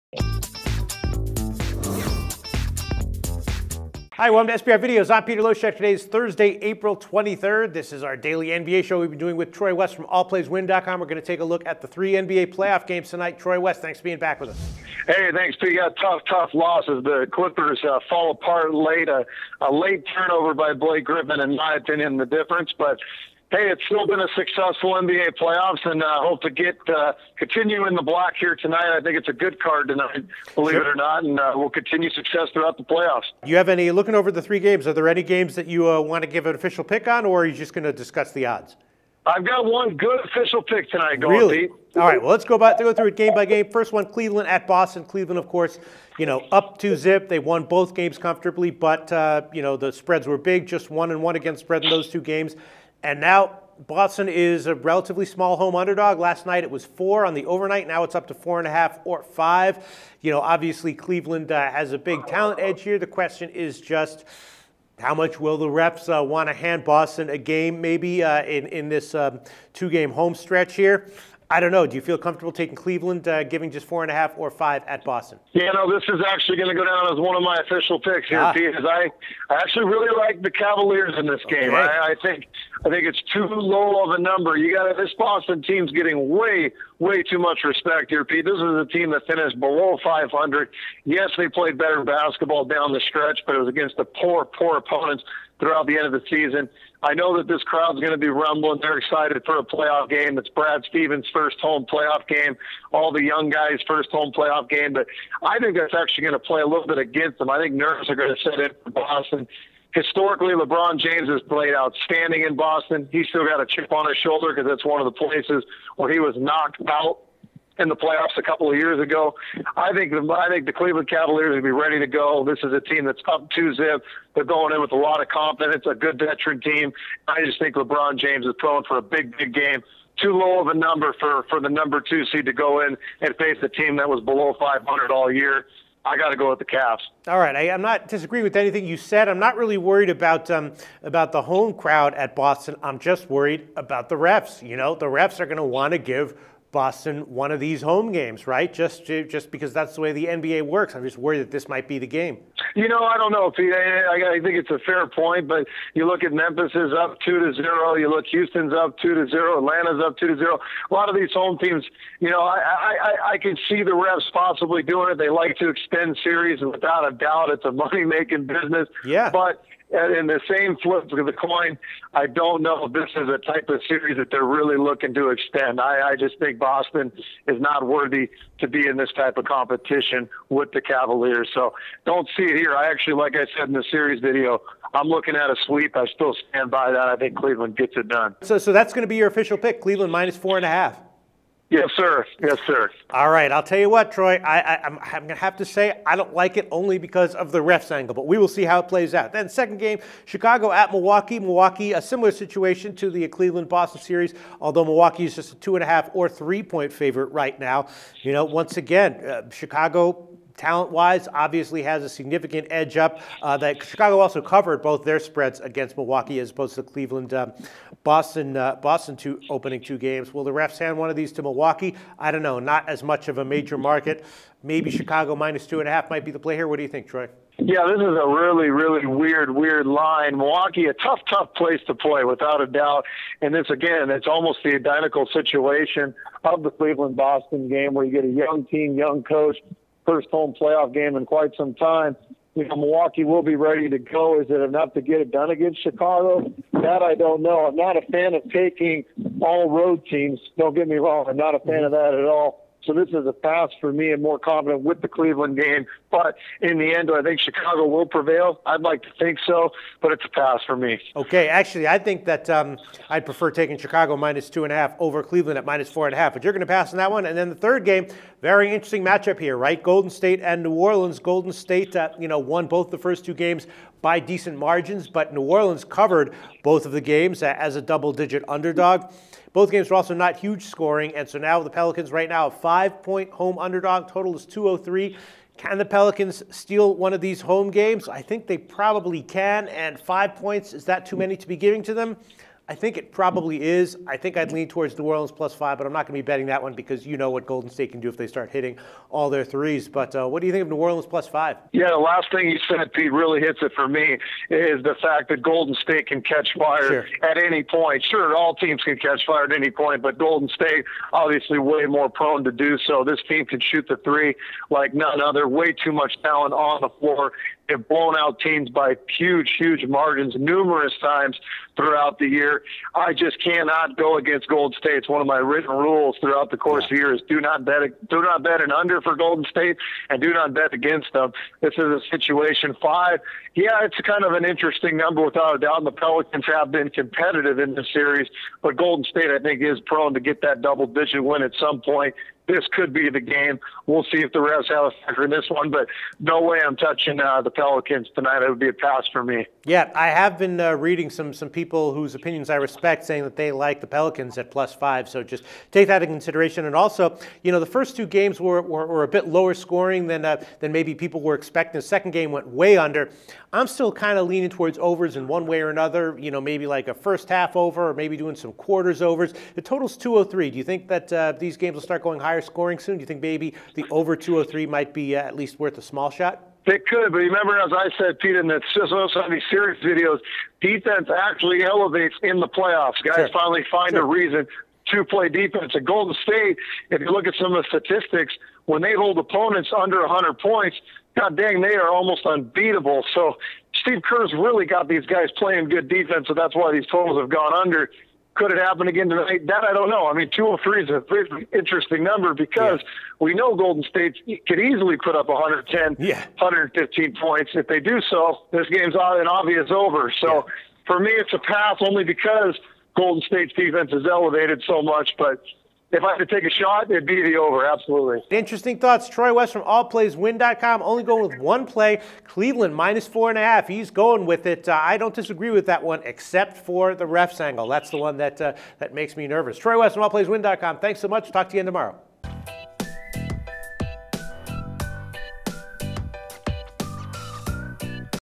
NBA Playoffs Picks 4/23 with Troy West, Loshak
Professional handicapper Troy West previews the NBA betting odds for the 3 NBA playoffs games for April 23rd with SBR Videos host Peter Loshak.